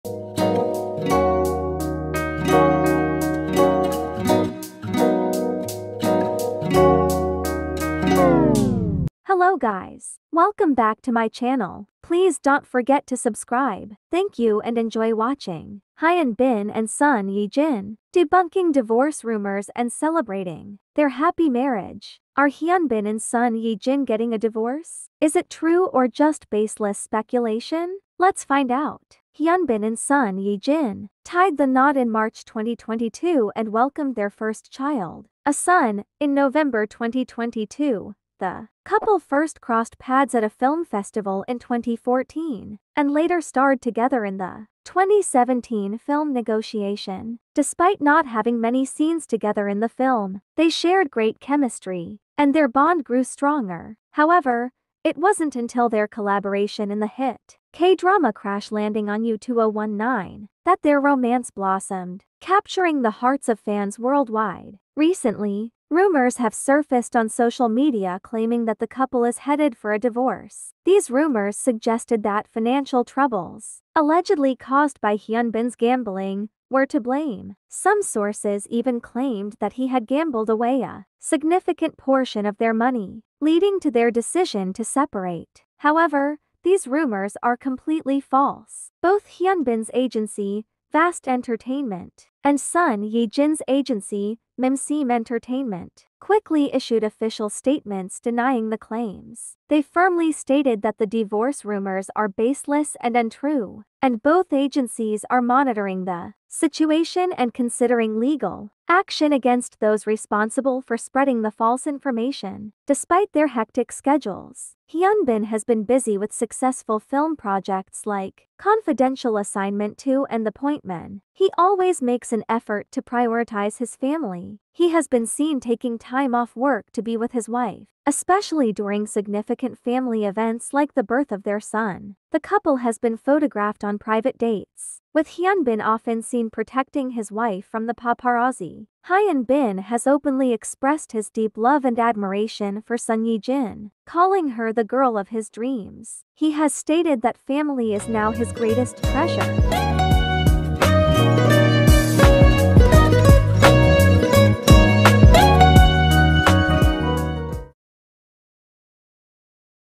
Are HyunBin and Son Ye-jin getting a divorce?Is it true or just baseless speculation?Let’s find out.
photo, audio, video not mine! only the edit